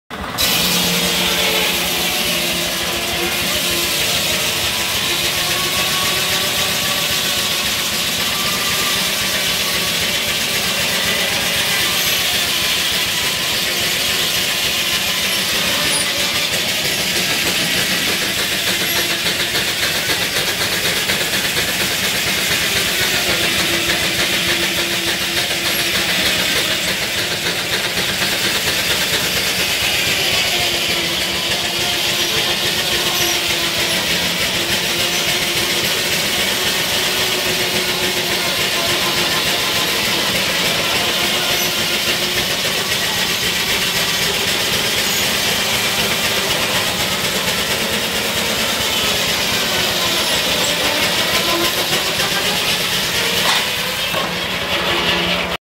Sawmill powered by Antique Steam Powered Tractor. ~ video short~
At the Miami Valley Steam Thresher's Reunion 2021.